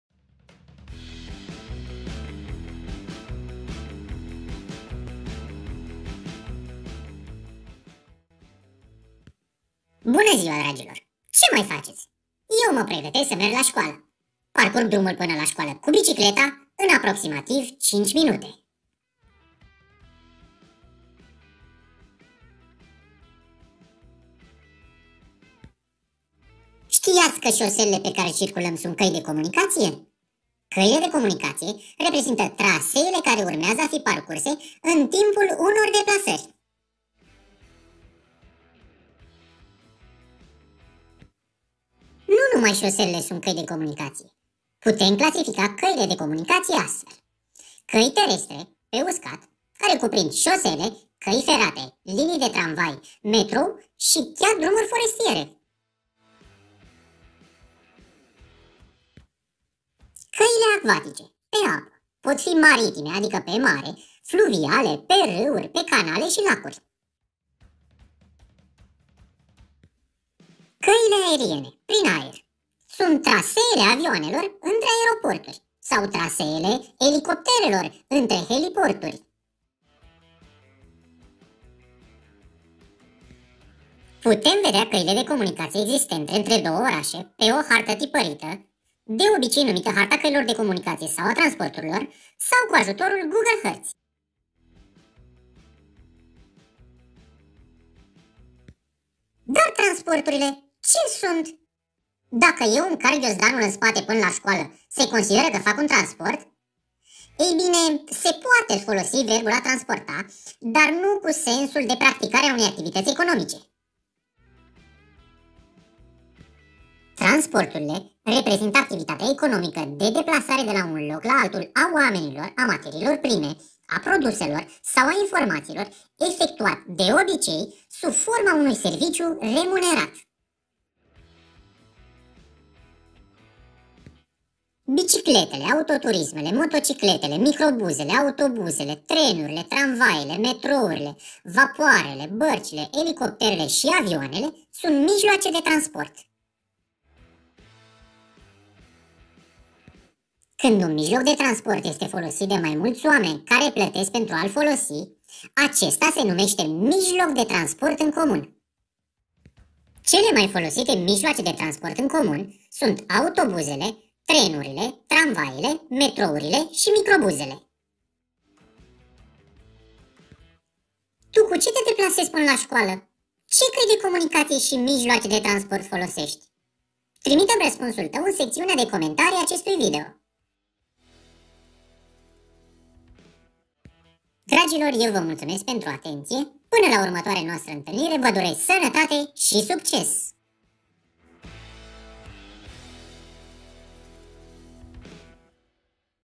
Căile de comunicație
Această resursă educațională deschisă își propune dezvoltarea competenței specifice 1.3. Utilizarea termenilor geografici simpli în contexte cunoscute, utilizând metoda explorării informațiilor video.
Cu ajutorul acestei resurse educaționale deschise, elevii își formează competența specifică mai sus menționată într-un mod plăcut, fiindu-le expuse informații audio-video referitoare la căi de comunicații și mijloace de transport.
La finalul acesteia le este propusă o temă de reflecție cu ajutorul căreia își pot fixa cunoștințele dobândite și pot continua demersul de formare al competenței specifice vizate.